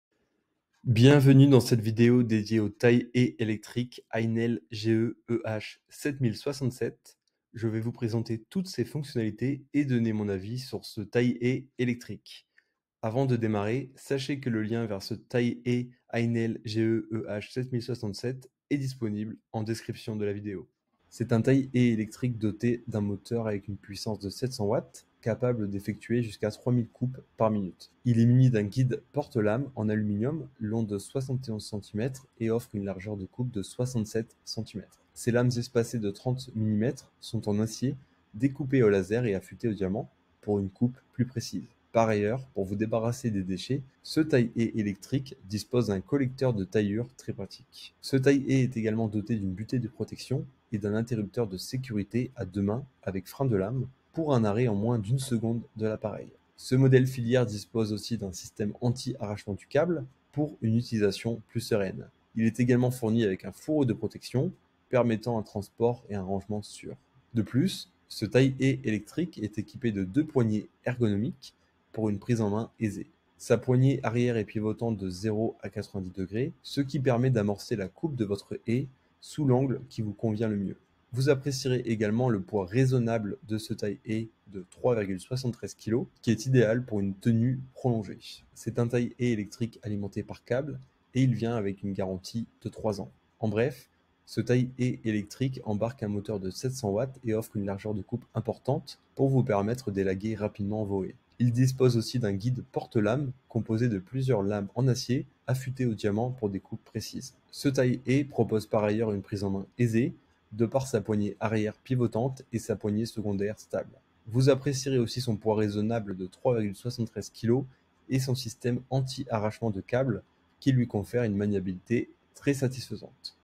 Taille haie électrique Einhell GE-EH 7067 : Mon avis complet
⭐ Taille-haie électrique Einhell GE-EH 7067

Dans cette vidéo je fais une présentation complète du taille haie électrique Einhell GE-EH 7067

Le juste choix participe au programme Amazon Partenaires.